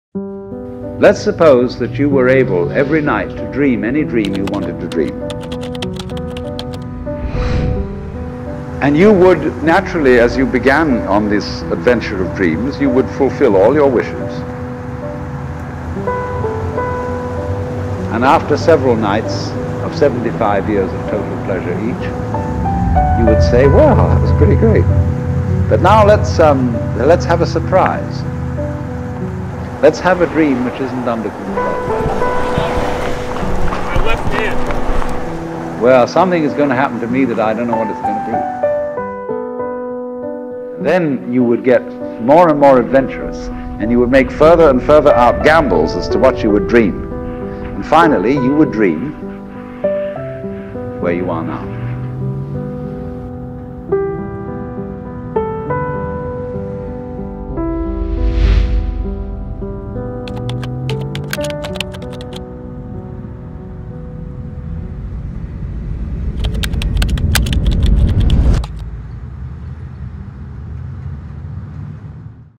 Creatify | Sunset Chasin' | Sony a7iii
hanging out with the Creatify boys and came out with this little sequence